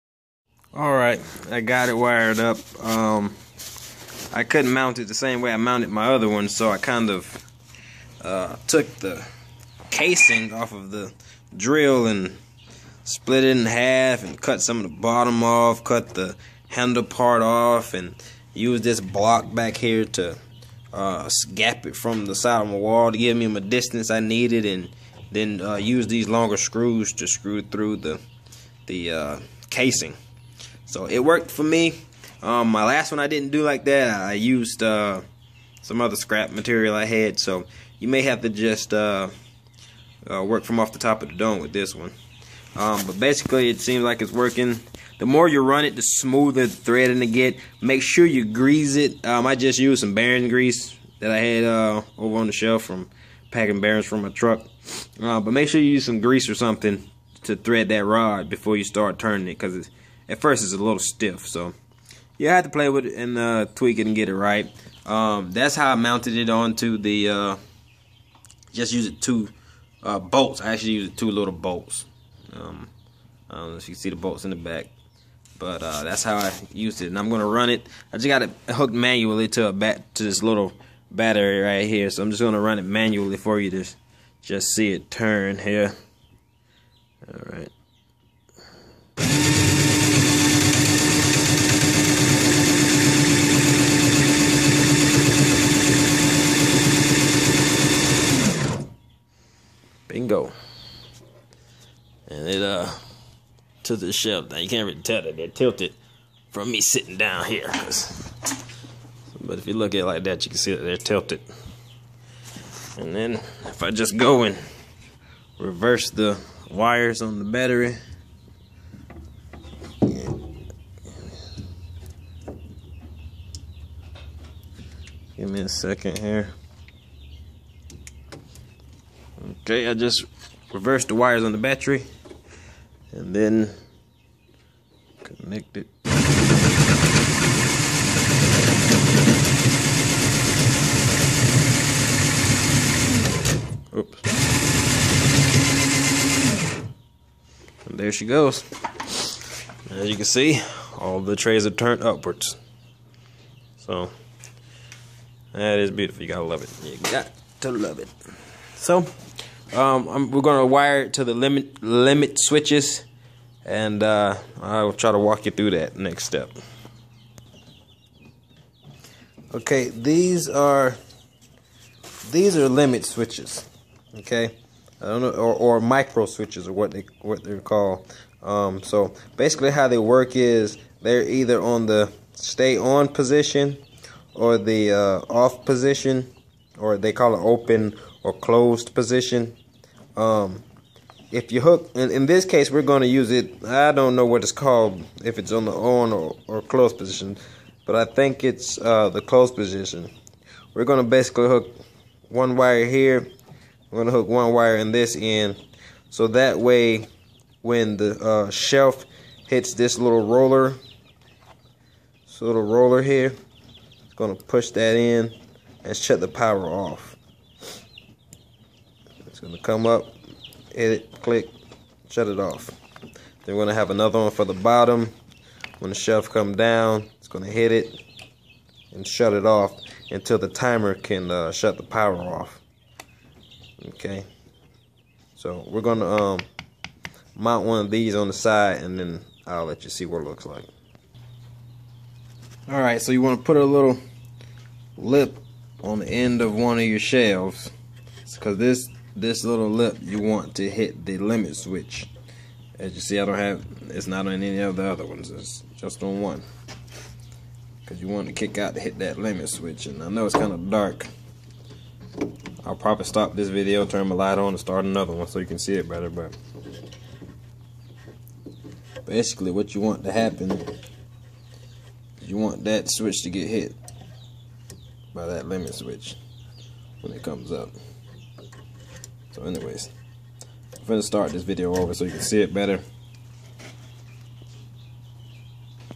Pt14 homemade incubator with automatic egg turner
Part 14 step by step video on how to convert a refrigerator into a incubator with automatic egg turner